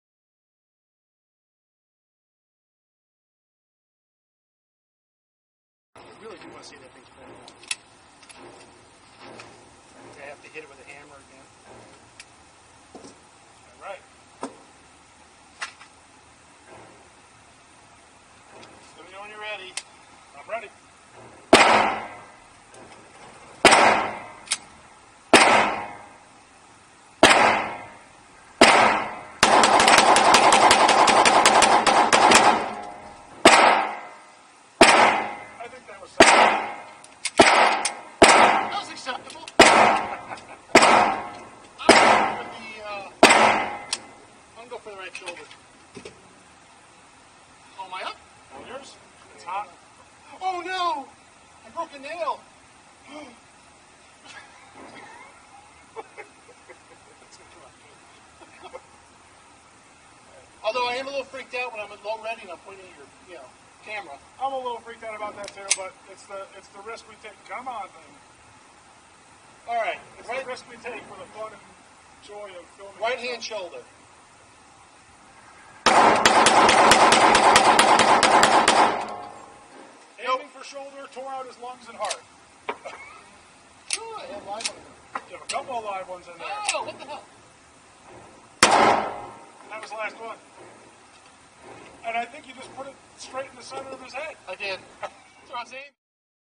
Beretta Brigadier (Lou Breaks a Nail), 9MM, Me and Lou, Fox Valley Shooting Range
Lou breaks a nail.